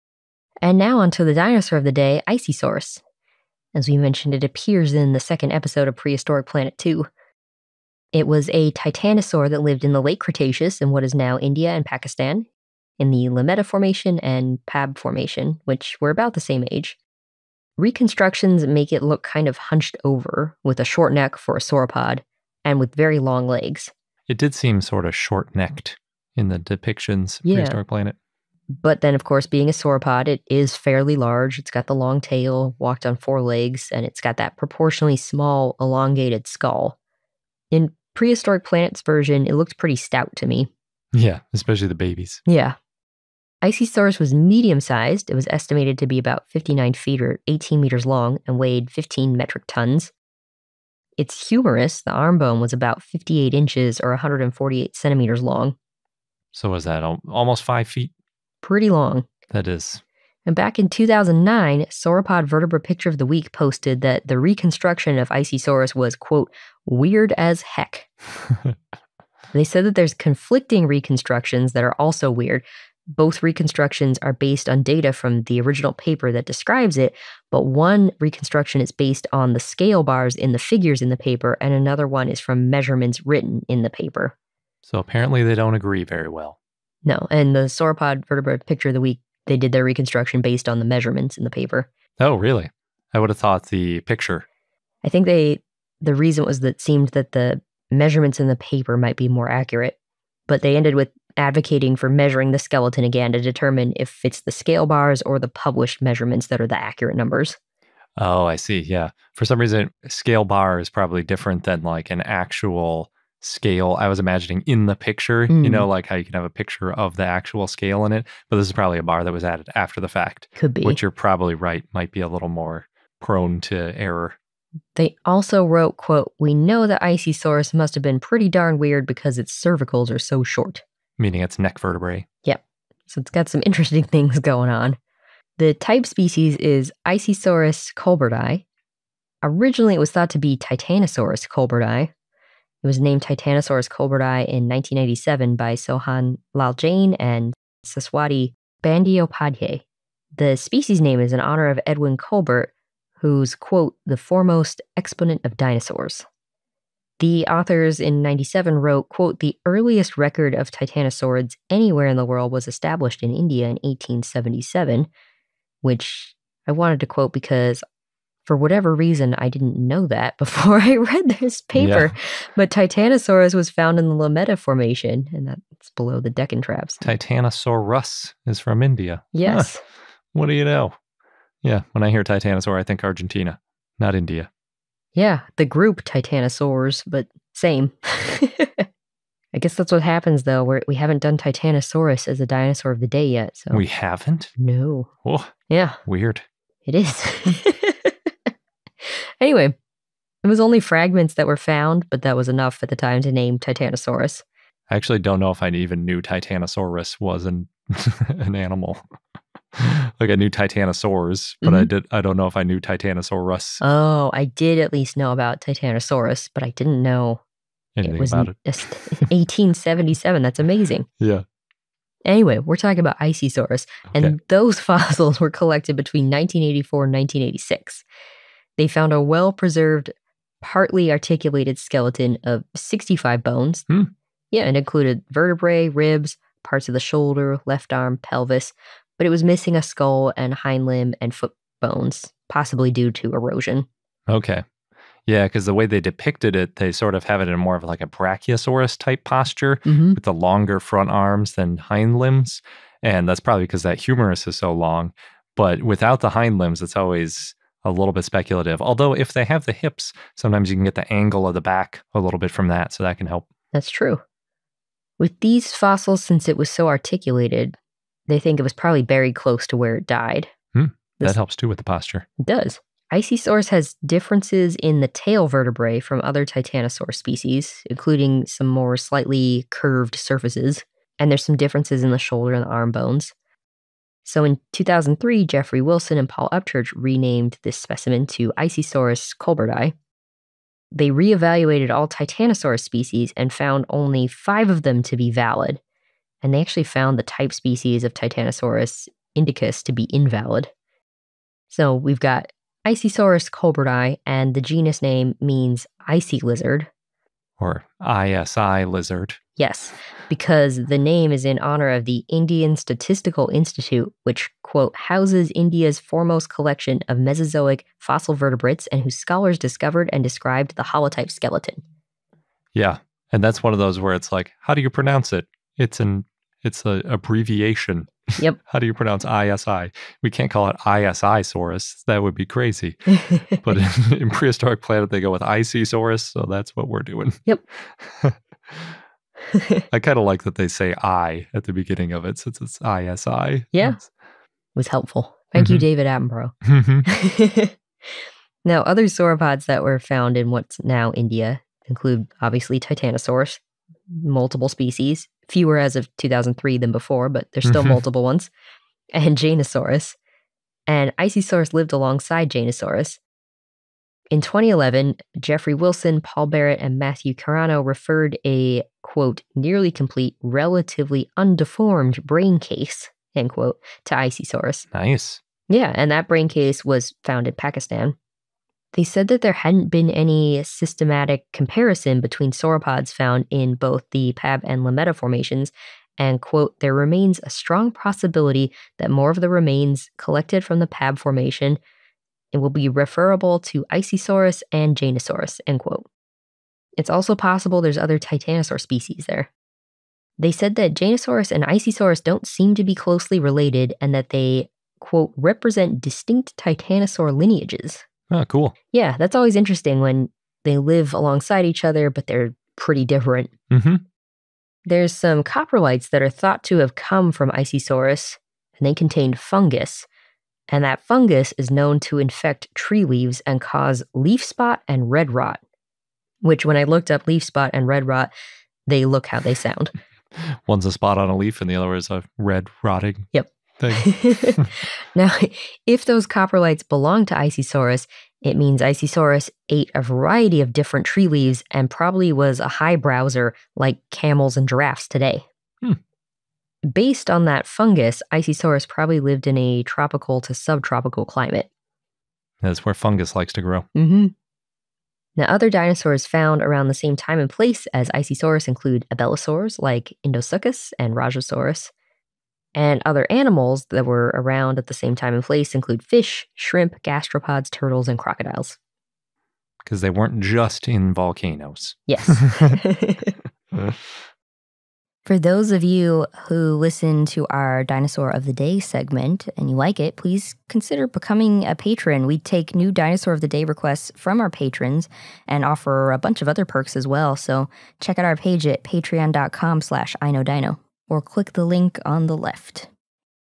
Isisaurus: Dinosaur of the Day
For the full episode where this dinosaur of the day was originally featured check out

Titanosaur that lived in the Late Cretaceous in what is now India and Pakistan (Lameta Formation and Pab Formation, which were about the same age)
Reconstructions make it look kind of hunched over, with a short neck for a sauropod, with very long legs
But like other sauropods, in that is fairly large, long tail, walks on four legs, has a proportionally small, elongated skull
In Prehistoric Planet, pretty stout
Medium-sized, estimated to be about 59 ft (18 m) long and weighed 15 metric tons
Humerus was about 58 in (148 cm) long
In 2009, Sauropod Vertebra Picture of the Week posted that the reconstruction of Isisaurus was "weird as heck"
Said that there are conflicting reconstructions that are also weird (both reconstructions based on data from the original paper, though one is from the scale bars in the figures in the paper and one is from measurements written in the paper)
Advocated measuring the skeleton again to determine if the scale bars or the published measurements are accurate
Wrote: "We know that Isisaurus must have been pretty darn weird because its cervicals are so short"
Type species is Isisaurus colberti
Originally thought to be Titanosaurus colberti
Named Titanosaurus colberti in 1997 by Sohan Lal Jain and Saswati Bandyopadhyay
Species name in honor of Edwin Colbert “foremost exponent of dinosaurs”
Sohan Jain and Saswati Bandyopadhyay wrote: “The earliest record of titanosaurids anywhere in the world was established in India in 1877” (Titanosaurus found in the Lameta Formation, which is below the Deccan Traps)